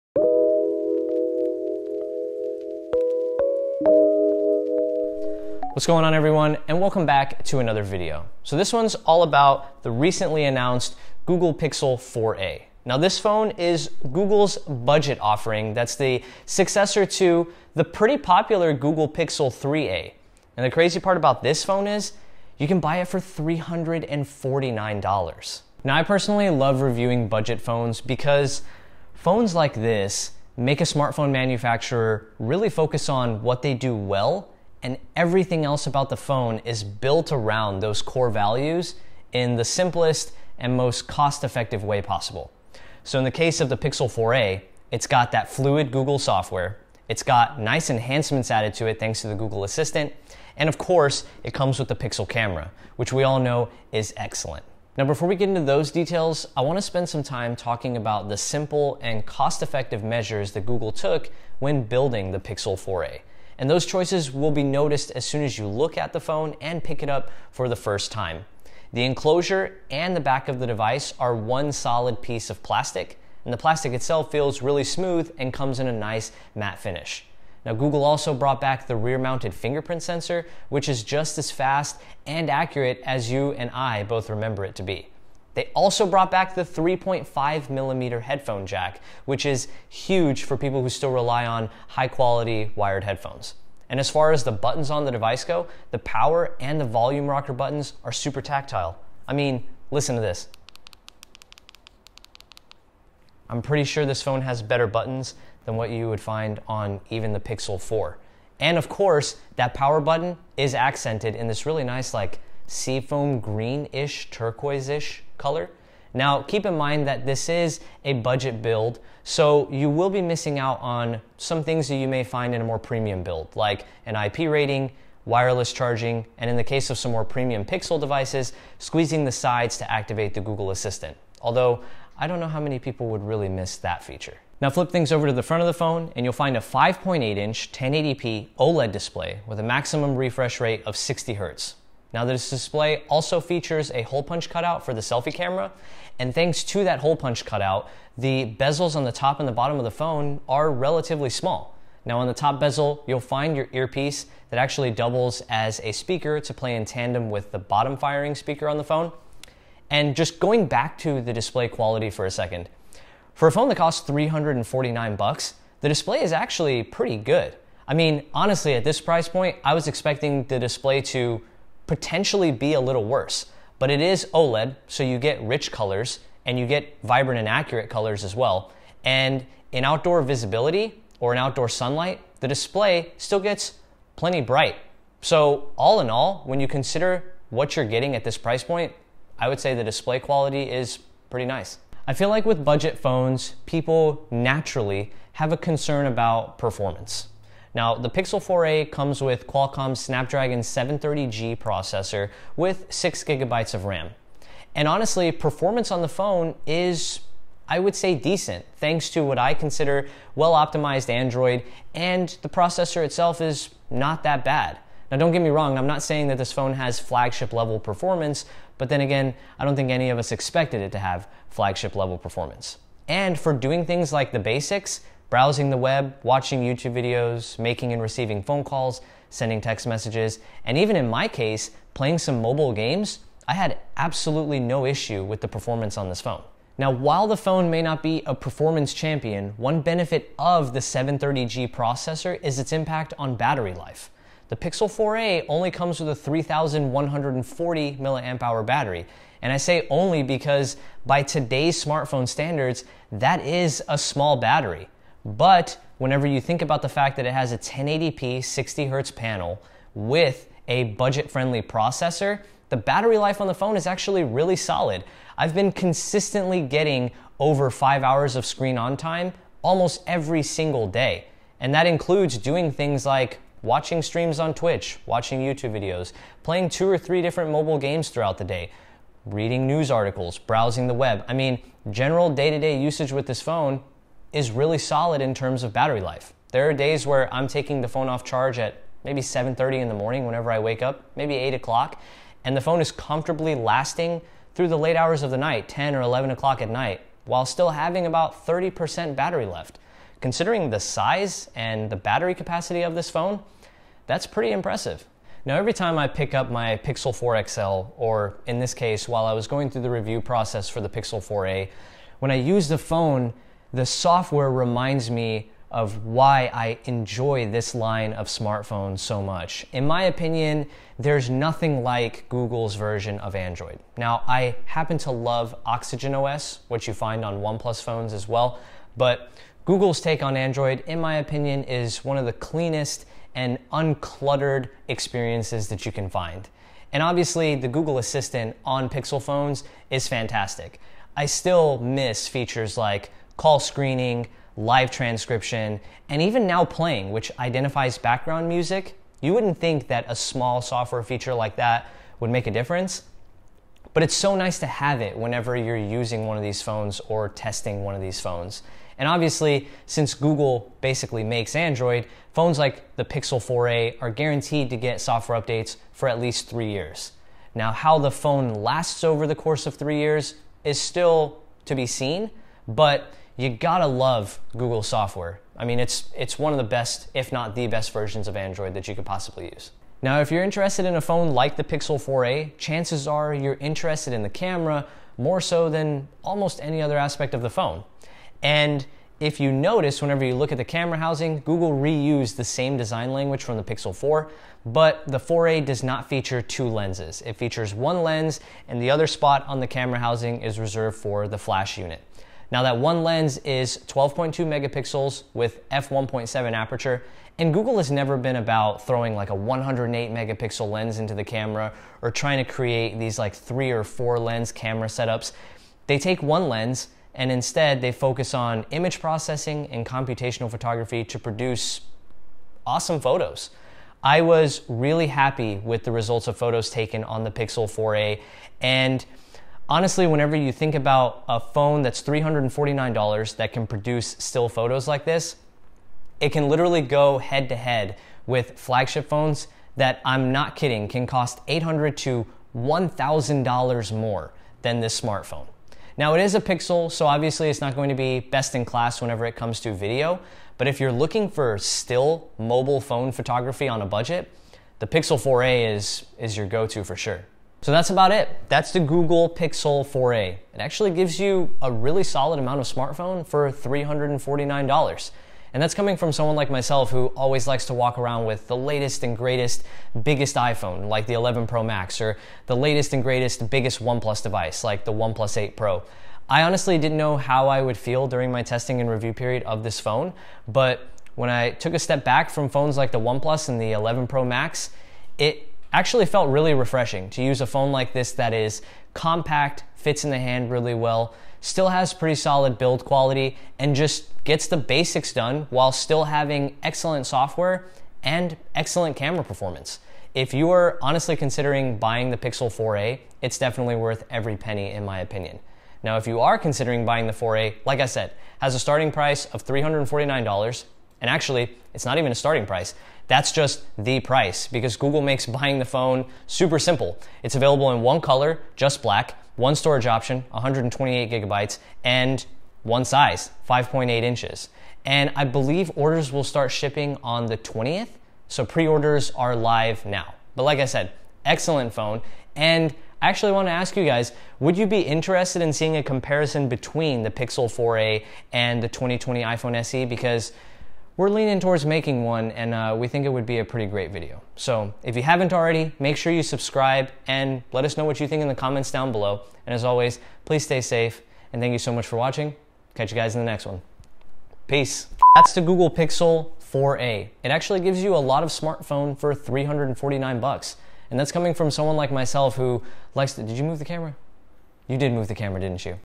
Google Pixel 4a Review - The best budget phone?
Hey everyone!

In this video we talk about the Google Pixel 4a, the latest budget offering from Google. It comes with fluid Android, the Google Assistant, and the Pixel camera, all for $349! But of course the question still remains, just how good is the Pixel 4a? It just might be the best budget phone of 2020...we got into more detail in the video!